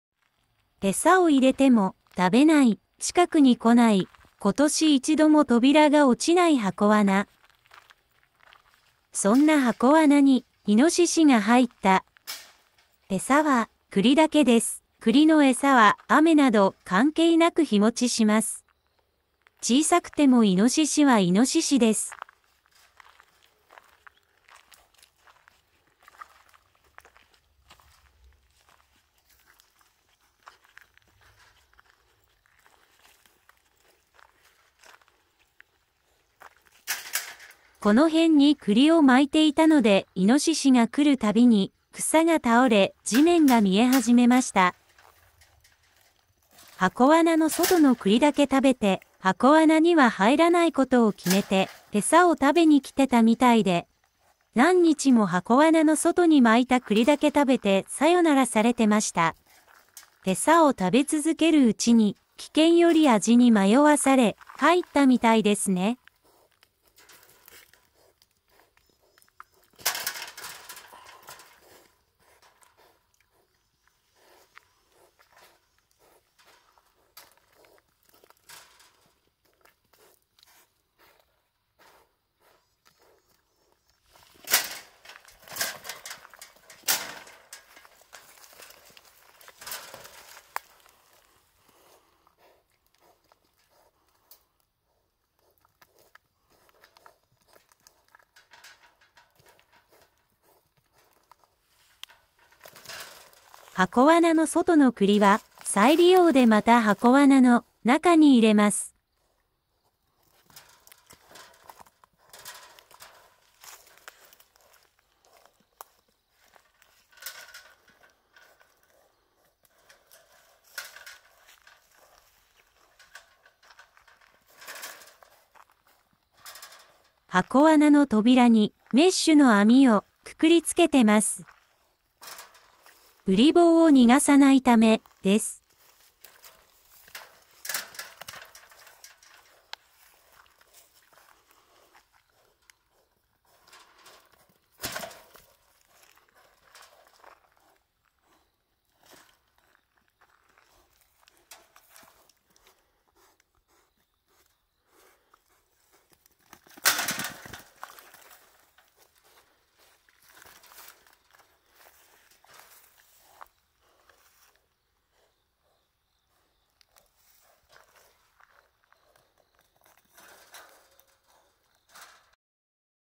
絶望からの脱出
今年一度も落ちない箱罠(正確に言えば今日の栗の時期に入りそれ以降は餌を入れても扉が落ちてません。)銃で猪を捕獲する人が減り山に猟犬を
入れないので猪も逃げる事なくその辺で生活してるので一度も箱罠に入ったのを見てるせいか箱罠には入りづらくなりました。11月からの猟期が始まると猪も猟犬に追われ移動が多くなり期待がする。